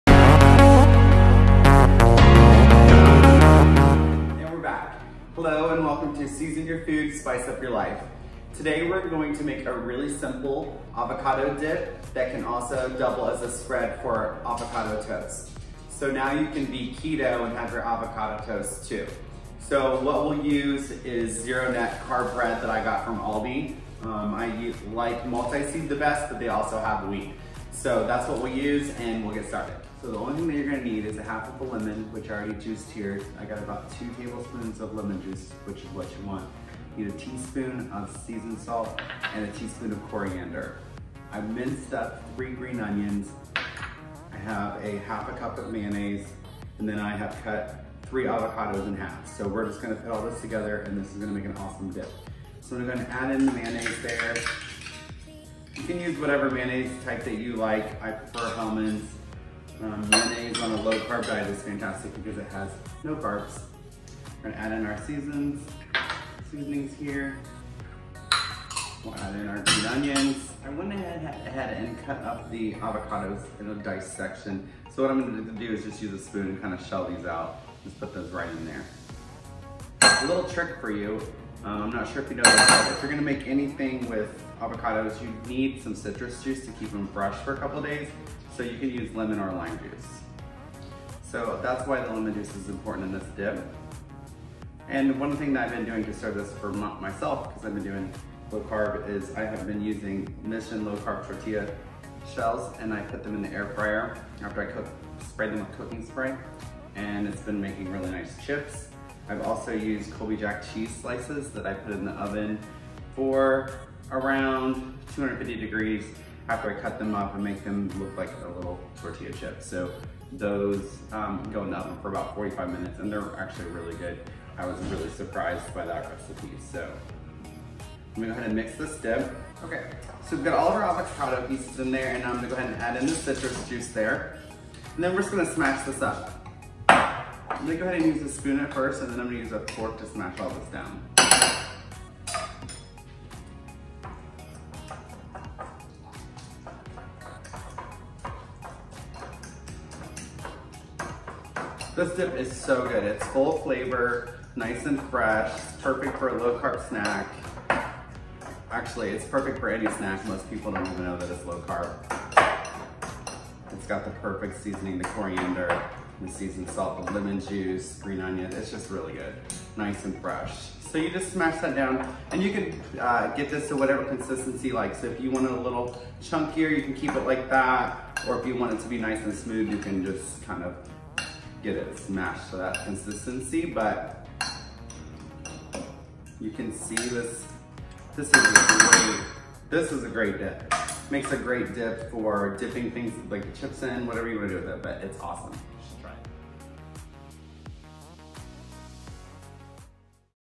Avocado Spread / Dip - Keto + Low Carb!
This amazing spread is so easy to make!  It’s fresh and tastes great as your avocado toast or dip!  It’s also great as a dip for those not doing low carb!  Full of flavor!!  Try it today!  You will love it!  Yum!